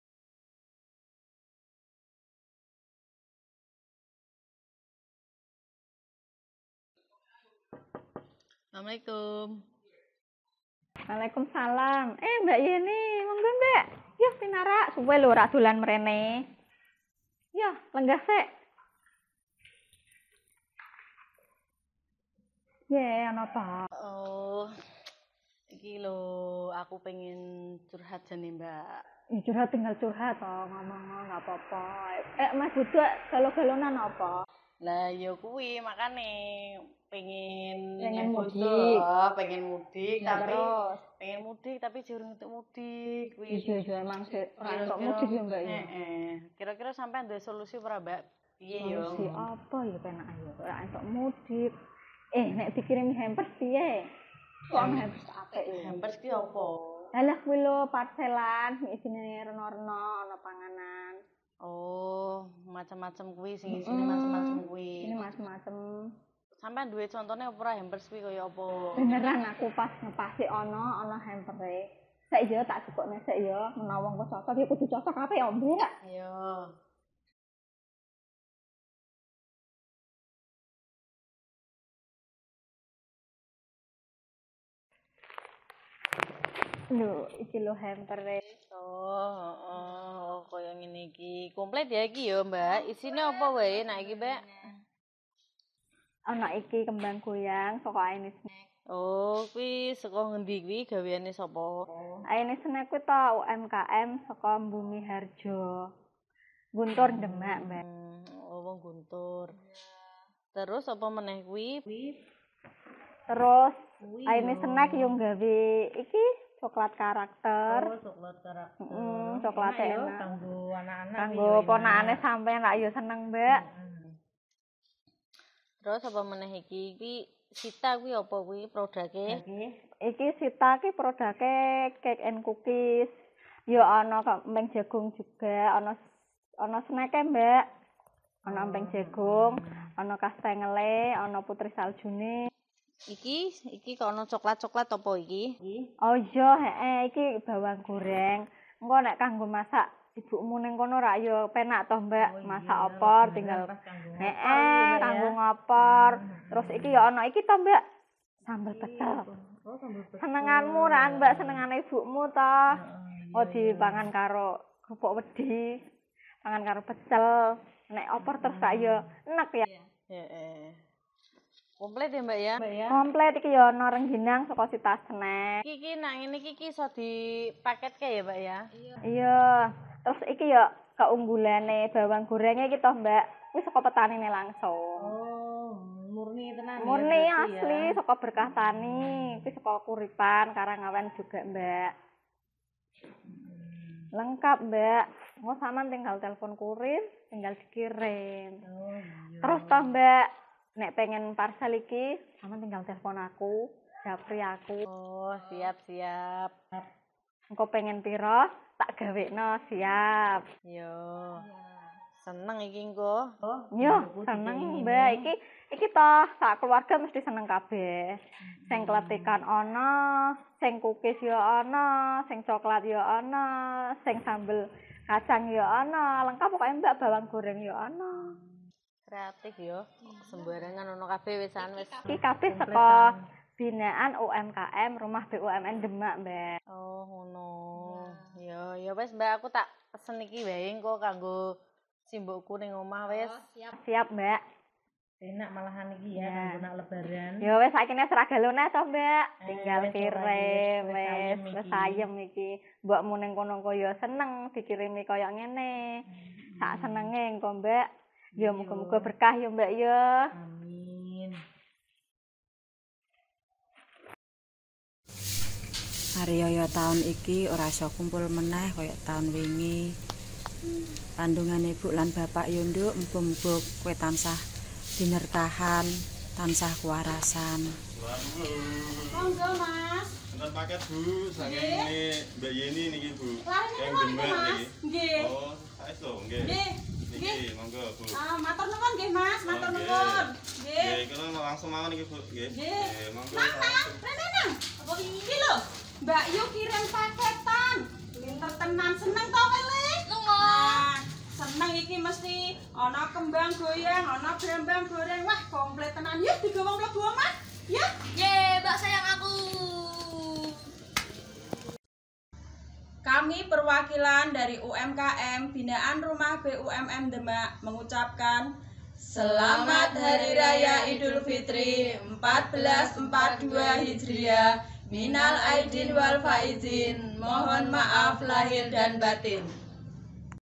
DEMAK #5 HAMPERS COMPETITION UMKM BRI –TIM Siti Masrofah
Hamper hasil kreasi dari Kelompok UMKM binaan Rumah BUMN BRI